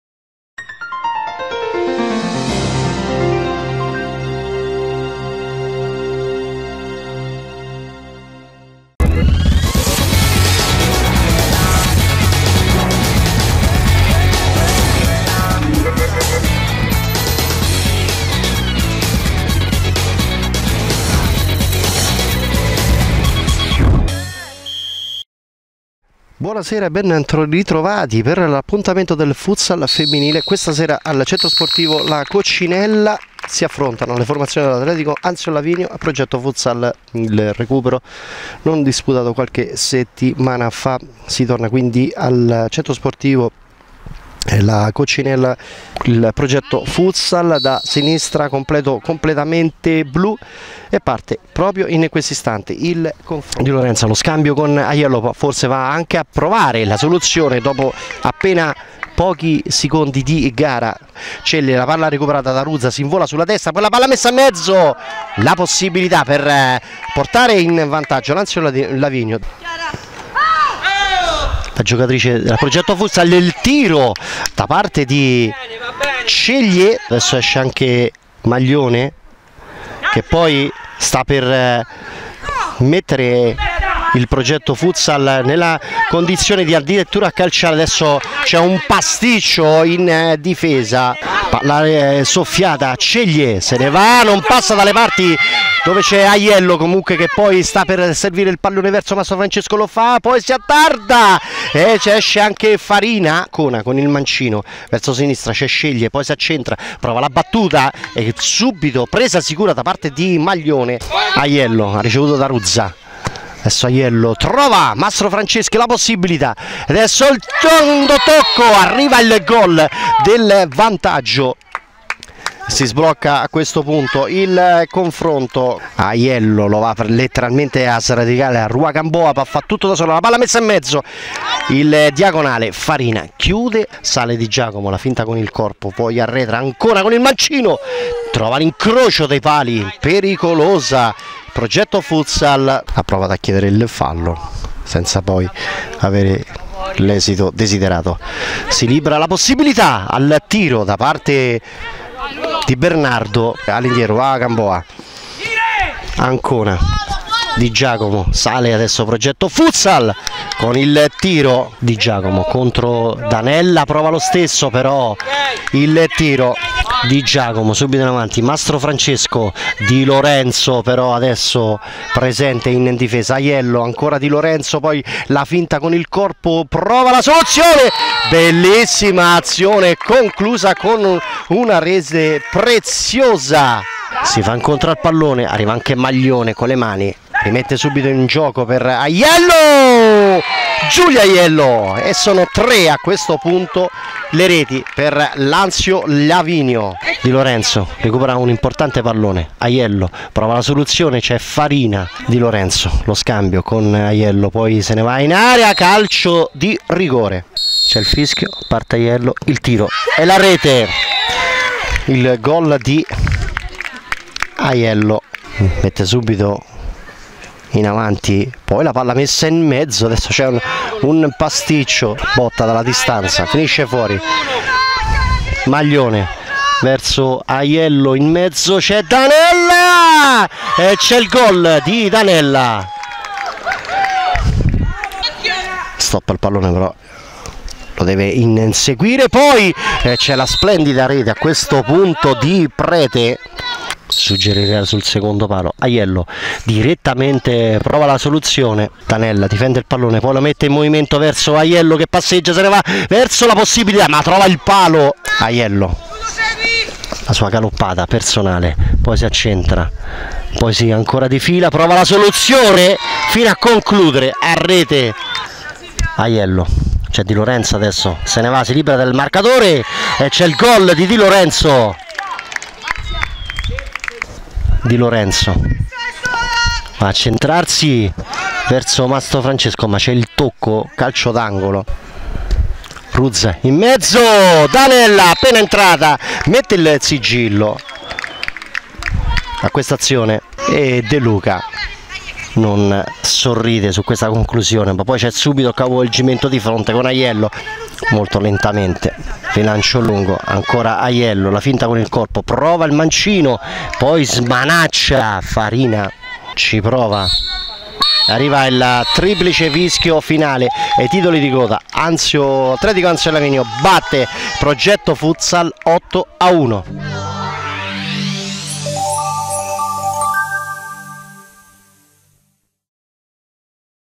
Serie D Femm.le: Atl. Anziolavinio vs Progetto Futsal - highlights
Serie D Femminile gir.A, 24a giornata: recupero della partita non disputatata in precedenza vede ancora una vittoria dell'Anziolavinio per 8-1